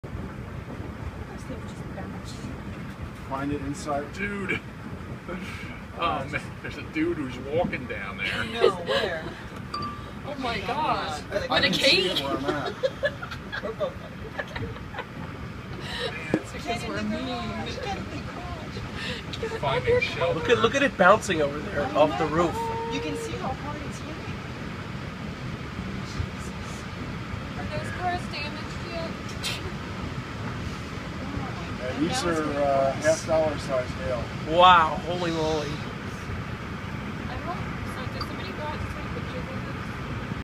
Hail
Hail in Reading, Pa., on May 22.